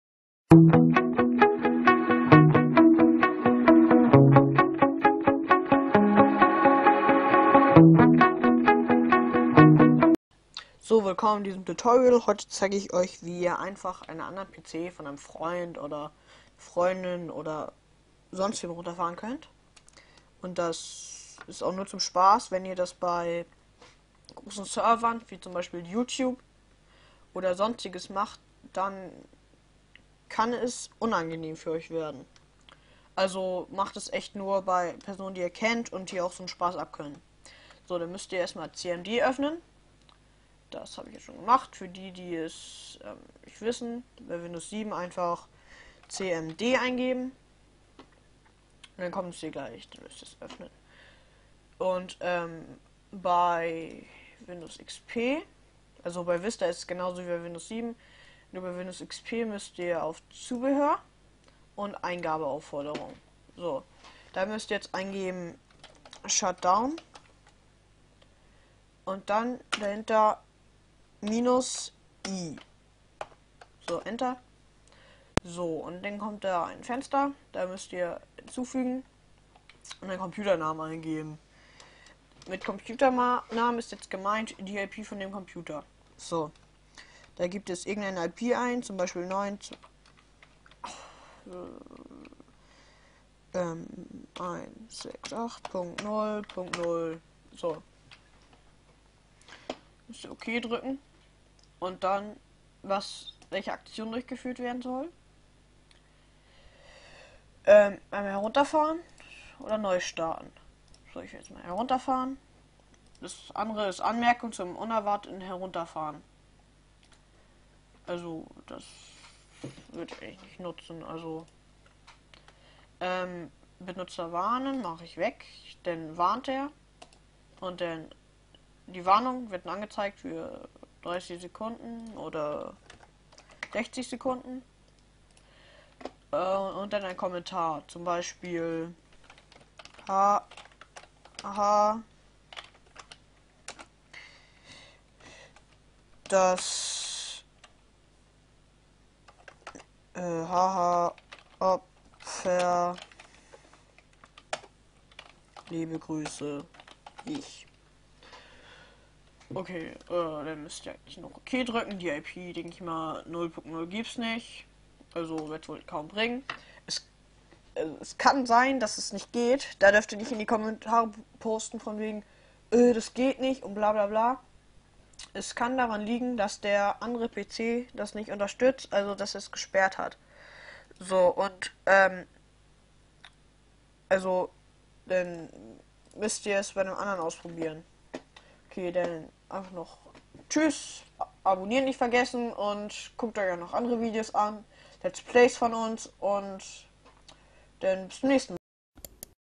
CMD-Einen anderen PC runterfahren
Ein Tutorial von Leif.
Code: shutdown -i

Euer Epic GAme Freaks Team